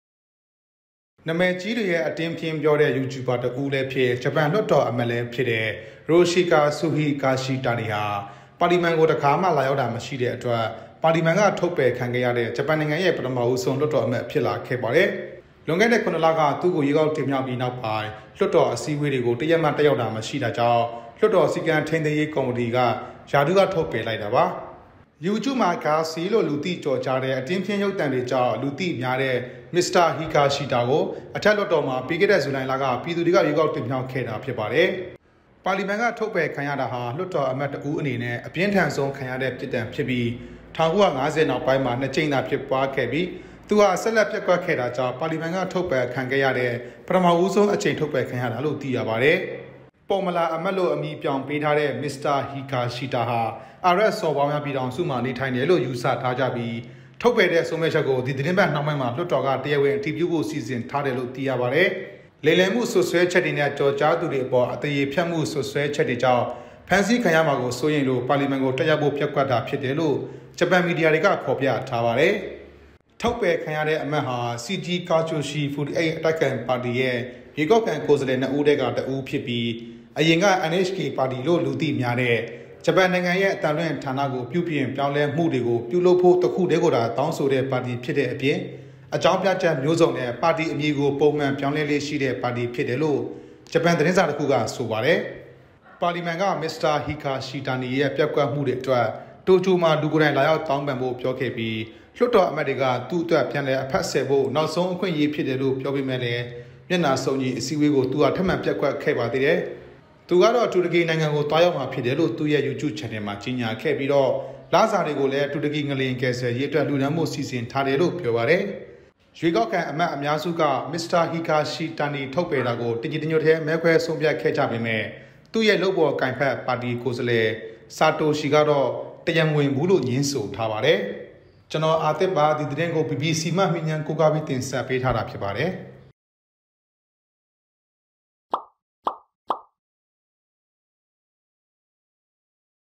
TLT Tv||- ပါလီမန်ကိုတစ်ခါမှလာရောက်တာမရှိတဲ့ ဂျပန် လွှတ်တော်အမတ်ရာထူးက ထုတ်ပယ်ခံရ
နာမည်ကြီး အတင်းအဖျင်းပြောတဲ့ ယူကျူဘာတစ်ဦးဖြစ်တဲ့ ဂျပန်လွှတ်တော်အမတ် ရိုရှီကာ ဆူဟီဂါရှီတာနီဟာ ပါလီမန်ကို တစ်ခါမှ လာရောက်တာမရှိတဲ့အတွက် ပါလီမန်က ထုတ်ပယ် ခံခဲ့ရတဲ့ ဂျပန်နိုင်ငံရဲ့ ပထမဆုံး လွှတ်တော်အမတ်ဖြစ်လာခဲ့ပါတယ်။